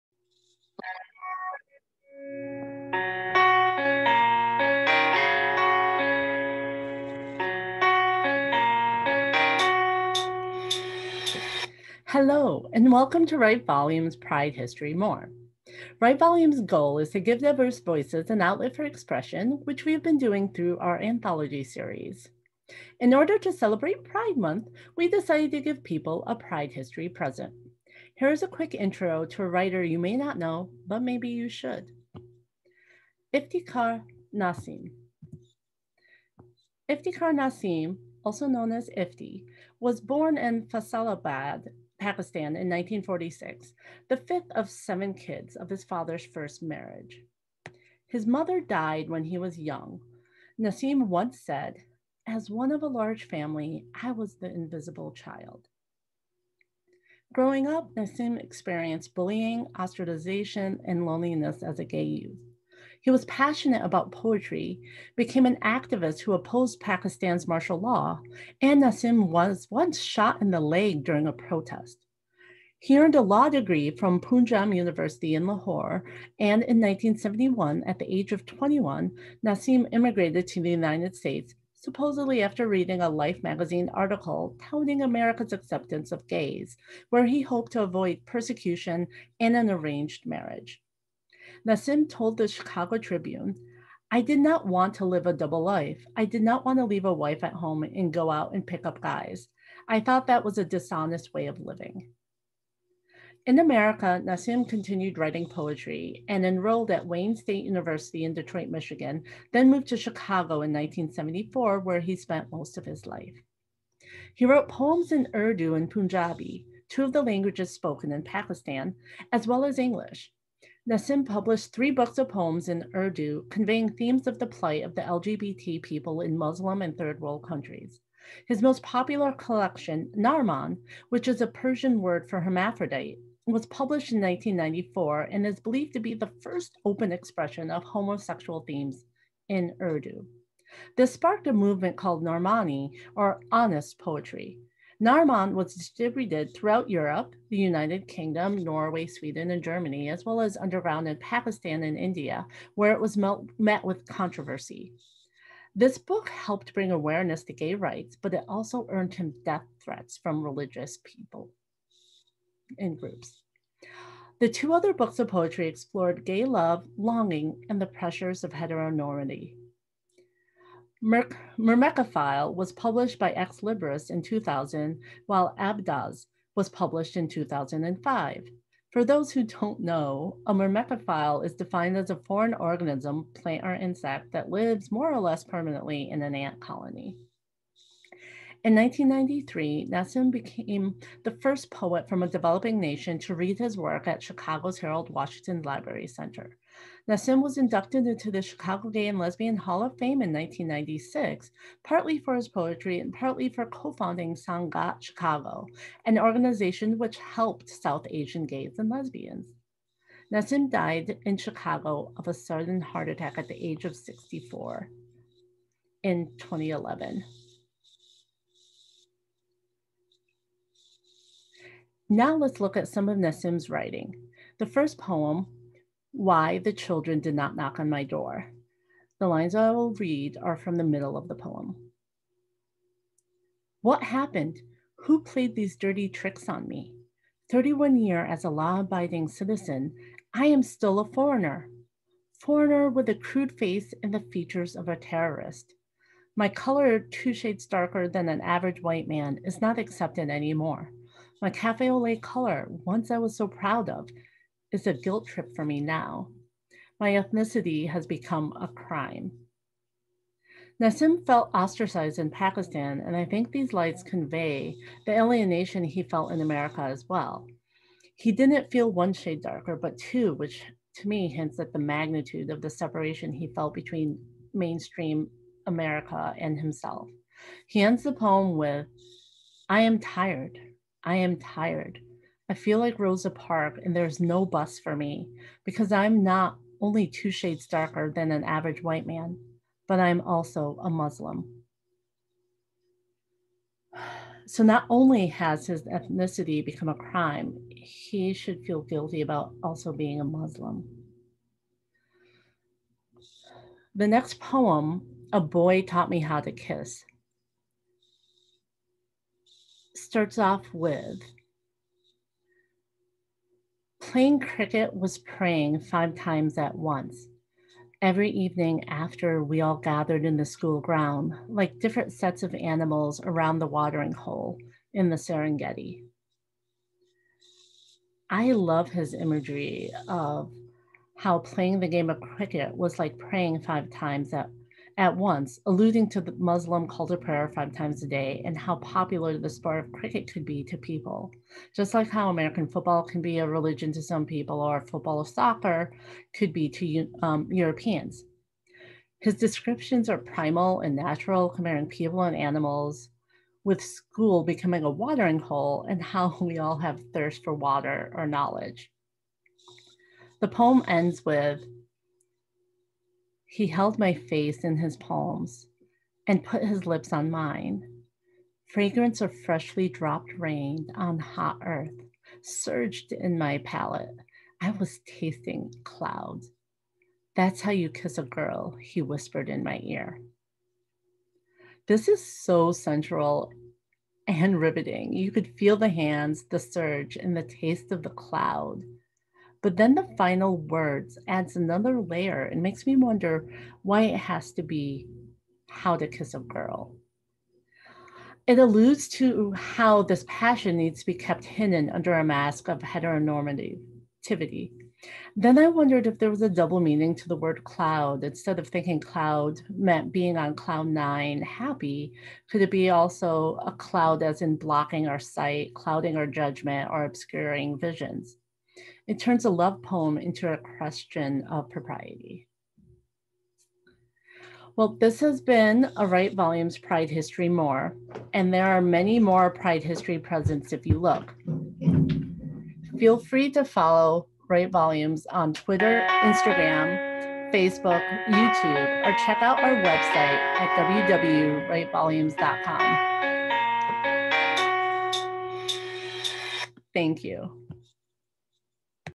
Write Volumes Celebrating Writers: Pride History More – Iftikhar Nasim
Welcome to Write Volumes Pride history more.  Write Volumes’ goal is to give diverse voices an outlet for expression which we have been doing through our anthology series. In order to celebrate Pride month, we decided to give people a Pride history present.

Here is a quick intro to a writer you may not know but maybe you should:
Iftikhar Nasim

Iftikhar Nasim also known as “Ifti” was born in Faisalabad, Pakistan in 1946 the fifth of seven kids of his father's first marriage. His mother died when he was young. Nasim once said, "As one of a large family I was the invisible child." Growing up, Nasim experienced bullying, ostracization and loneliness as a gay youth.  He was passionate about poetry, became an activist who opposed Pakistan’s martial law, and Nassim was once shot in the leg during a protest.  He earned a law degree from Punjab University in Lahore and in 1971, at the age of 21, Nassim immigrated to the United States supposedly after reading a Life magazine article touting America’s acceptance of gays where he hoped to avoid persecution and an arranged marriage.  Nasim told the “Chicago Tribune”: “I did not want to live a double life. I did not want to leave a wife at home and go out and pick up guys. I thought that was a dishonest way of living.” In America Nasim continued writing poetry and enrolled at Wayne State University in Detroit, Michigan then moved to Chicago, IL in 1974 where he spent most of his life.

He wrote poems in Urdu and Punjabi (two of the languages spoken in Pakistan) as well as English. Nasim published three books of poems in Urdu, conveying themes of the plight of LGBT people in Muslim and third-world countries. His most popular collection, "Narman" which is a Persian word for “hermaphrodite," was published in 1994 and is believed to be the first open expression of homosexual themes in Urdu.  This sparked a movement called narmani or "honest" poetry. "Narman" was distributed throughout Europe (United Kingdom, Norway, Sweden and Germany) as well as underground in India and Pakistan where it was met with controversy. The book helped bring awareness to gay rights, but it also earned him death threats from religious groups.

The two other books of poetry explored gay love, longing and the pressures of heteronormativity. “Myrmecophile" was published by Xlibris in 2000 while "Abdoz" was published in 2005.  For those who don’t know a myrmecophile is defined as a foreign organism (plant or insect) that lives more or less permanently in an ant colony.

In 1993 Nasim became the first poet from a developing nation to read his work at Chicago’s Harold Washington Library Center.  Nasim was inducted into the Chicago Gay and Lesbian Hall of Fame In 1996, partly for his poetry, and partly for co-founding Sangat/Chicago, an organization which helped South Asian gays and lesbians.

Nasim died in Chicago of a sudden heart attack at the age of 64 in 2011.

Now let’s look at some of Nasim’s writing.  First poem “Why the Children did not Knock on My door.” These lines are from the middle of the poem:

What happened? Who played
These dirty tricks on me?
Thirty one year as a law abiding citizen
I am still a foreigner. Foreigner
With a crude face and features of
a terrorist. My color two shade
Darker than an average white man
Is not accepted anymore.
My café ole color, once I was so proud of,
Is a guilt trip for me now.
My ethnicity has become a crime

Nasim felt ostracized in Pakistan and I think these lines convey the alienation he felt in America as well.  He didn’t feel one shade darker but 2 which to me hints at the magnitude of the separation he felt between mainstream American and himself.

The next poem “A Boy Taught me How to Kiss a Girl” ends with:

He held my face in his palms
And put his lips on mine.
Fragrance of freshly dropped rain on hot earth
Surged in my palate
I was tasting cloud.
“ That’s how you kiss a girl.”
He whispered in my ear.

This is so sensual and riveting you can feel the hands, the surge, and taste of the cloud but then the final words adds another layer and makes me wonder why it has to be a girl. It alludes to how this passion needs to be kept hidden under a mask of heteronormativity.  Then I wondered if there was a double meaning to the word cloud instead of thinking cloud meant being on cloud 9 happy, could it be cloud as in blocking our sight, clouding judgement, or obscuring visions.  It turns a love poem into a question of propriety.

There are many more Pride history presents if you look.

Feel free to follow Write Volumes on Twitter, Instagram, Facebook, Youtube or sign up for our email list on our

Please let us know if there are more writers you feel should be a part of our Celebrating Writing series by commenting below!